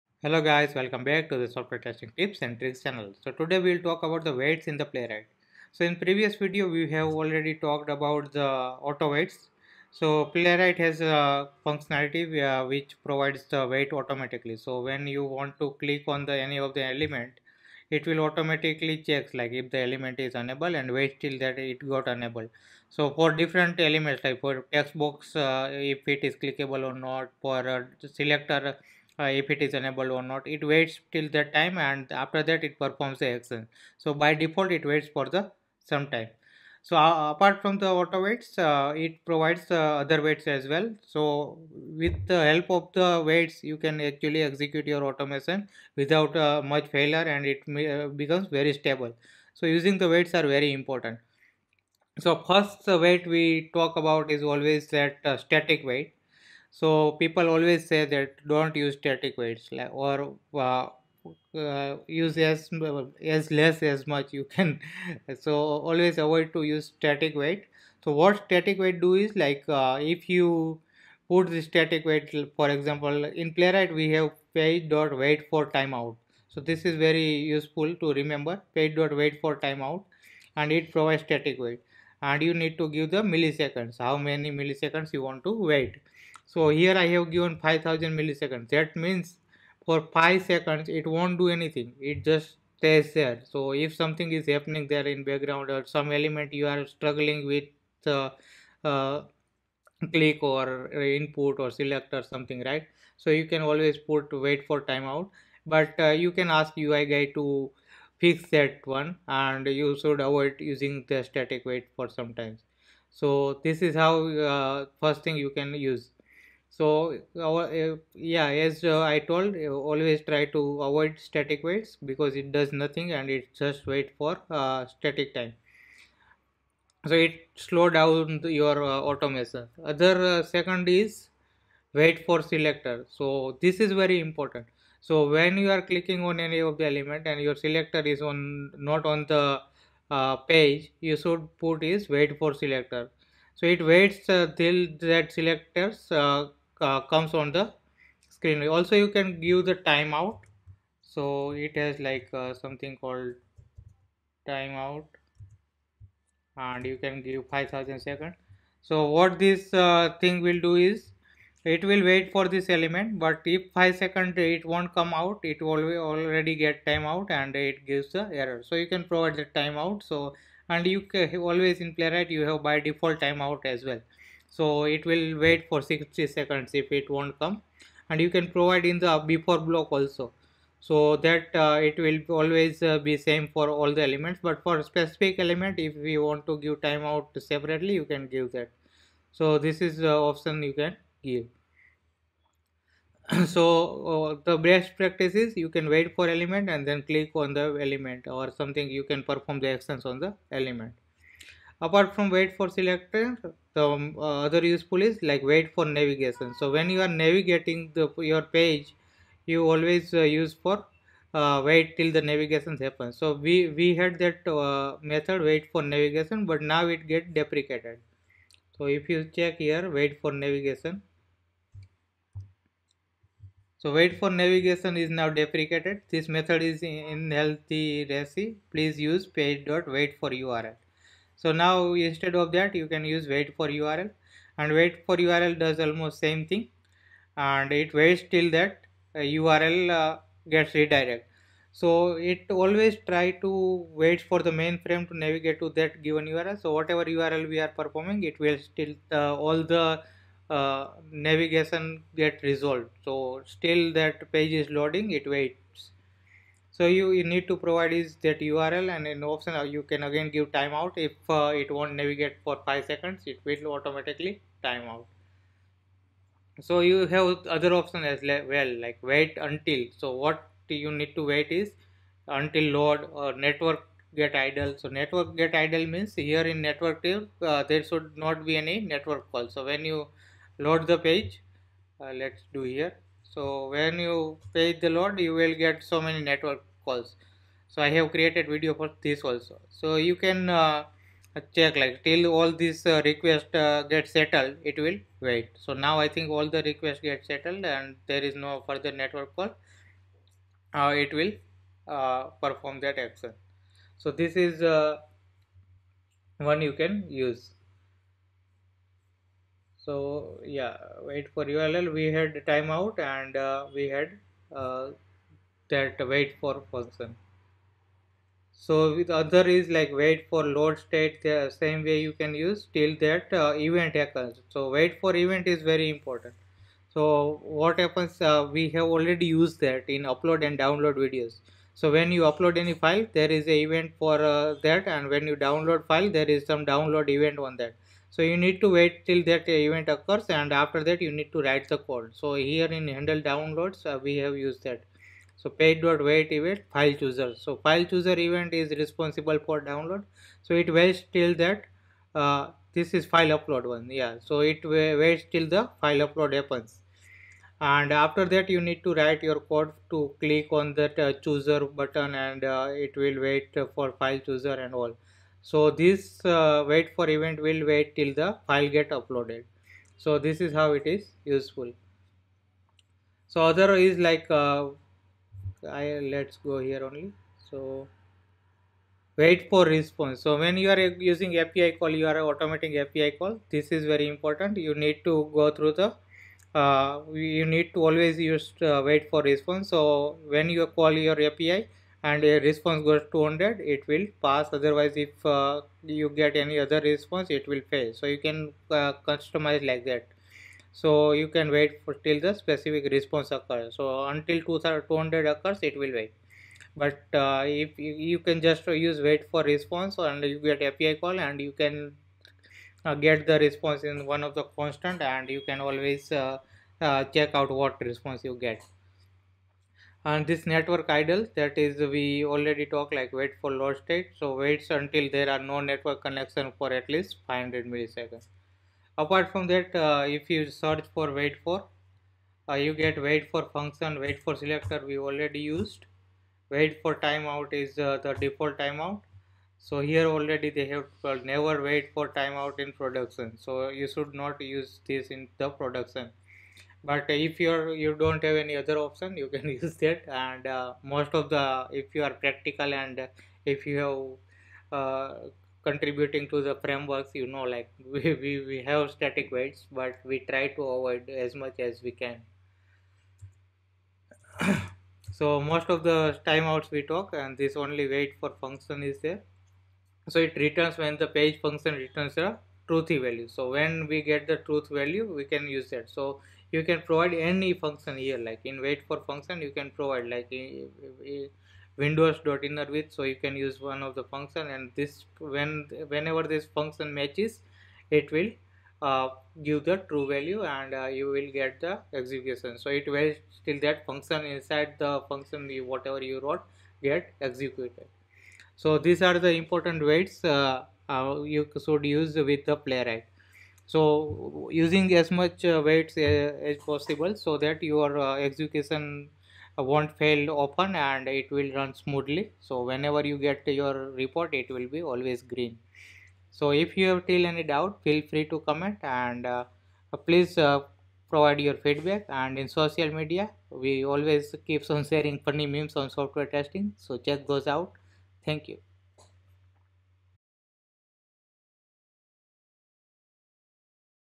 Playwright tutorials for beginners - How to use Waits in Playwright?
Waits are very important for any automation tool. Waits provide stability in automation execution. Playwright comes with the default auto waits which handles most of the element's actions. Along with auto wait playwrigh provides "waitForTimeout()", "waitForEvent()", "waitForURL()", "waitForSelector()", "waitForResponse()" etc. All these are methods provided in the page class.
In this video, I have explained "how to use waits in Playwright?". I have explained how and when to use static wait and other waits provided by the playwright.